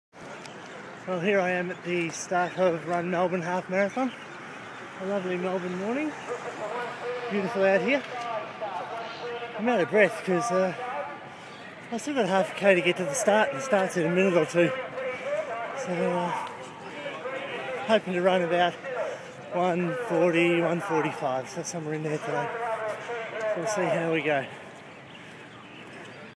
Run Melbourne 2010
PB's pre-race comments at the Run Melbourne Half Marathon. Little did I know the race had begun and I had already missed the start!